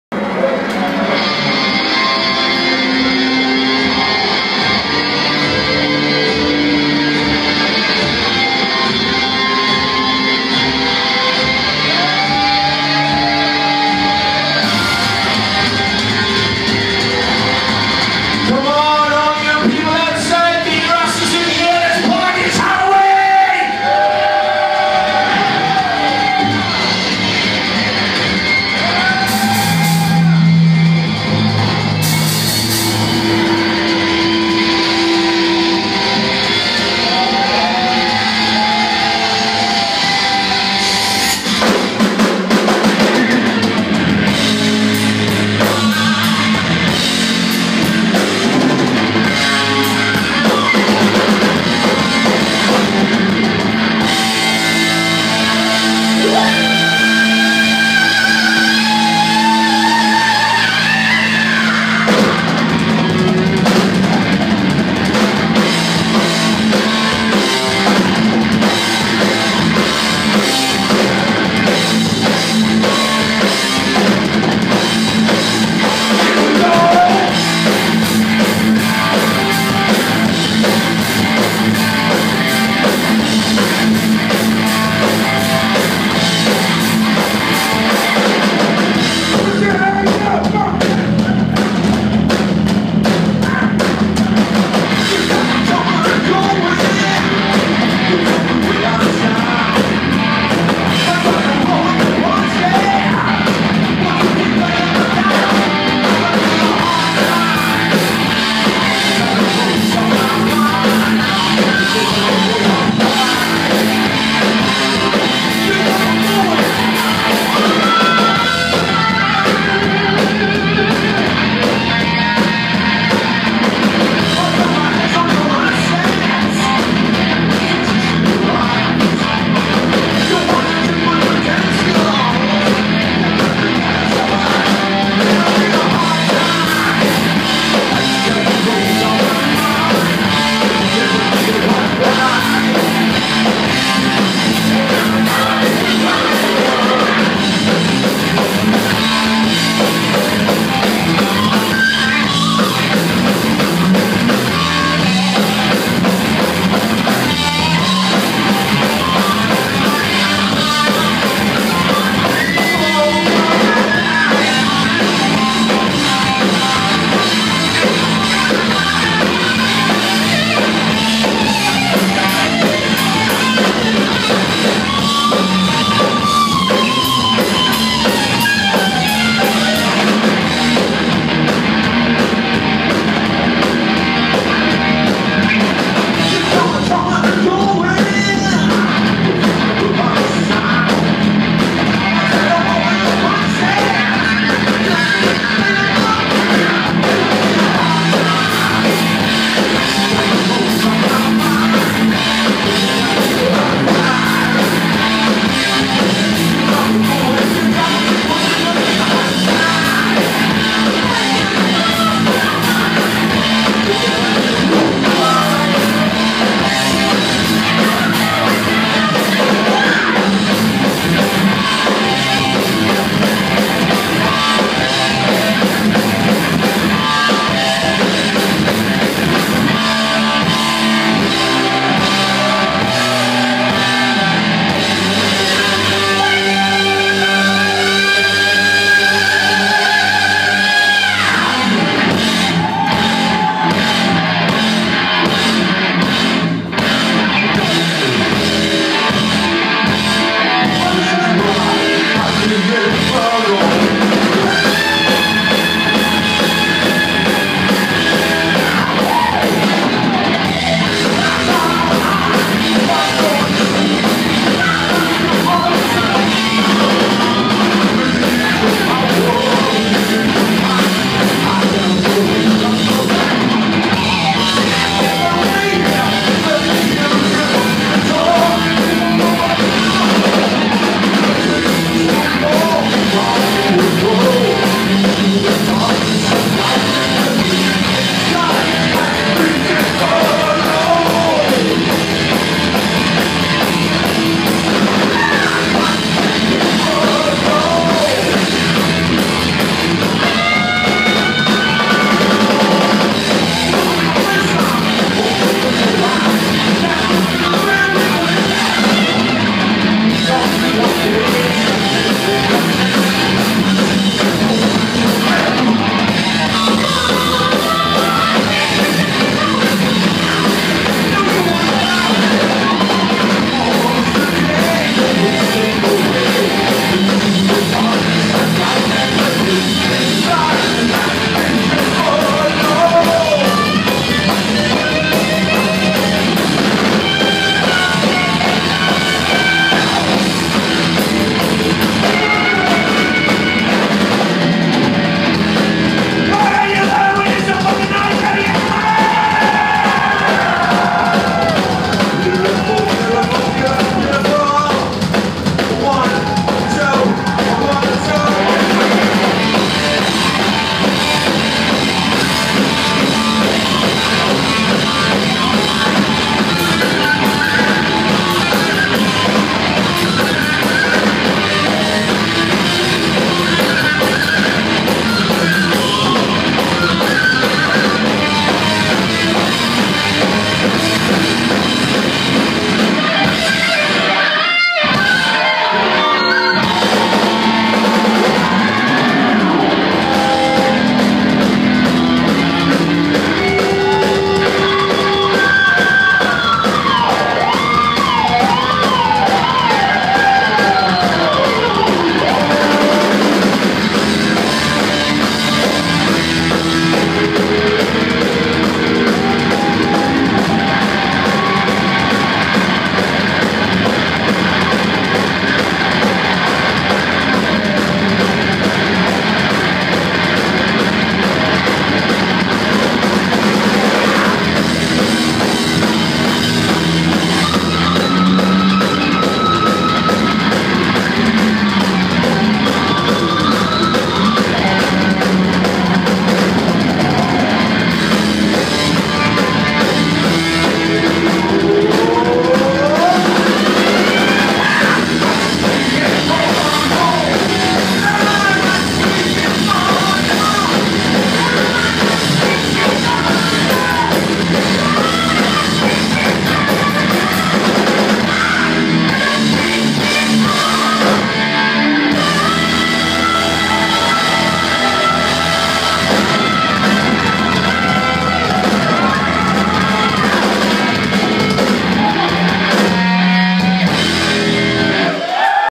RAVEN @ The Hop! 10/31/2014 #1
Loved seeing the live performance of RAVENS in Spokane, Washington at The Hop! Halloween 2014